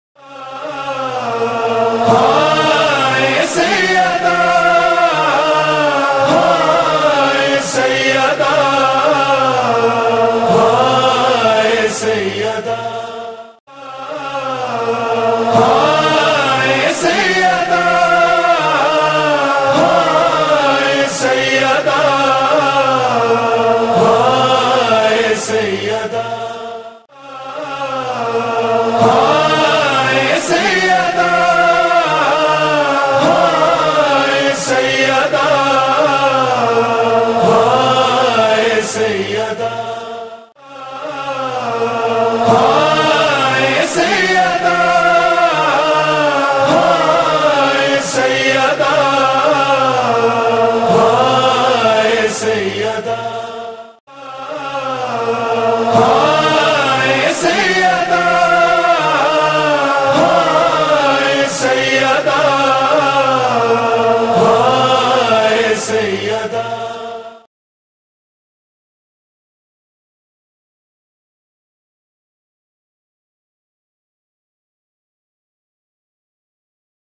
Majlis-o-Matami Jaloos Basilsila-e-Shahdat BB Fatima Zehra s.a
Majlis-o-Matami Jaloos Basilsila-e-Shahdat BB Fatima Zehra s.a held on 6th May 2011 at Imam Bargah Shohda-e-Karbala a.s Incholi Blk: 20 F.B.Area Karachi,
From: Tanzeem-e-Karwan-e-Najaf(reg)